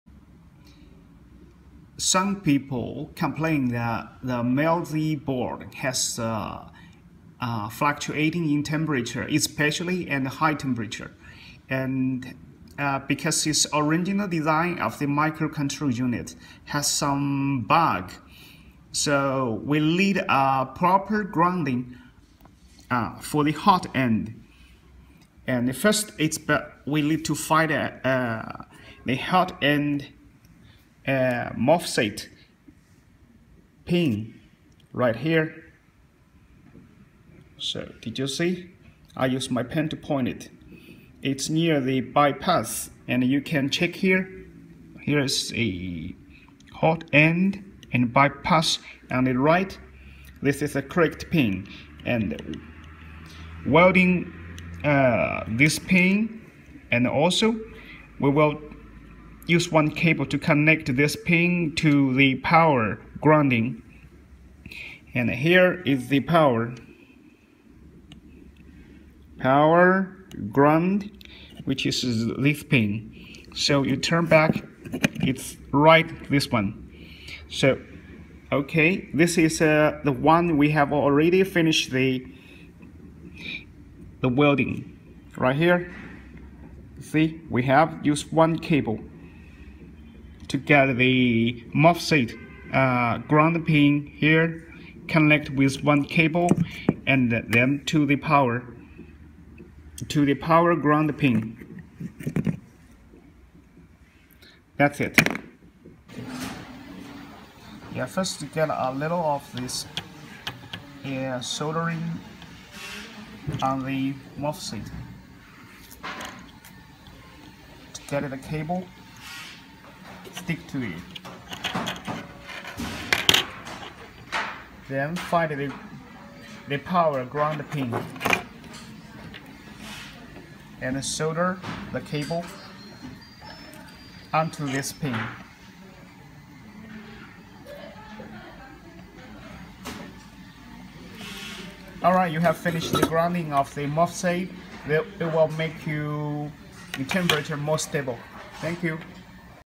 Wanhao Duplicator i3 extruder temperature flucturating solution
Wanhao i3 extruder temeperature may flucturate hugely.  So one cable soldering is required.